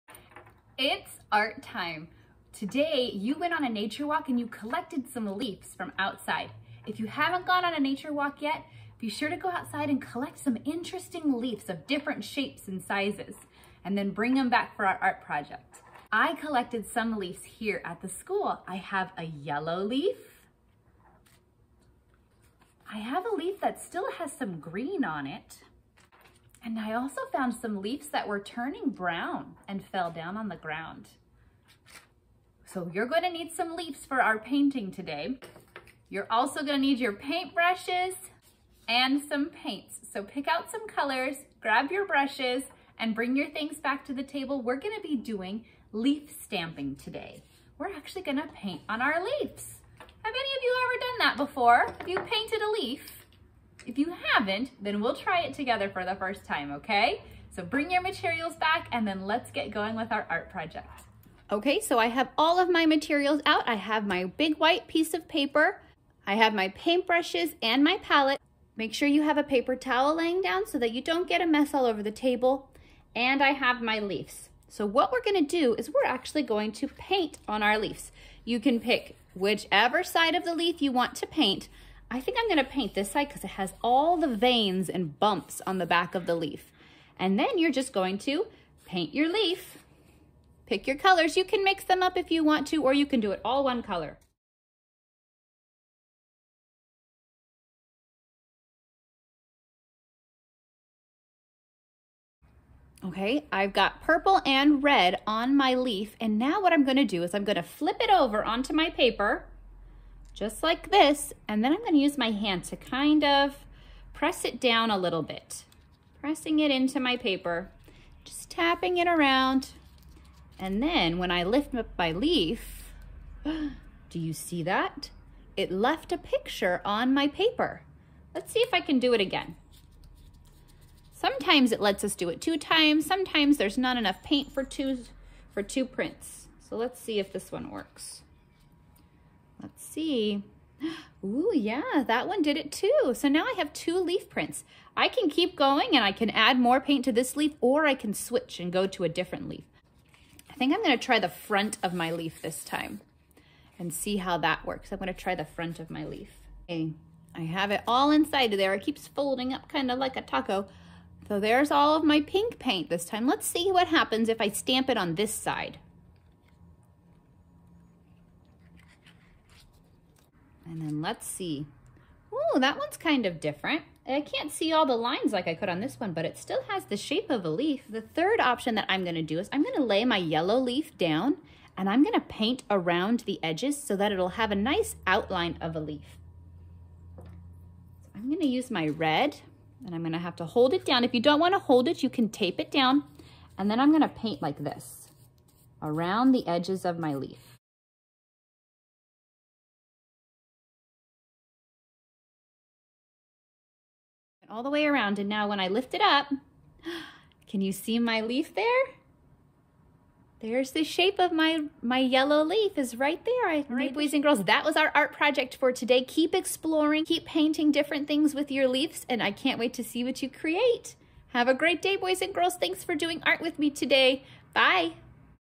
Leaf Printing Art Activity
We are exploring print making with leaves and paint today.

Materials:
-Tempera Paint
-Leaves

Modification: Use leaves and crayons without wrappers.  Put the leaf under your paper and rub the crayon over it to make an image.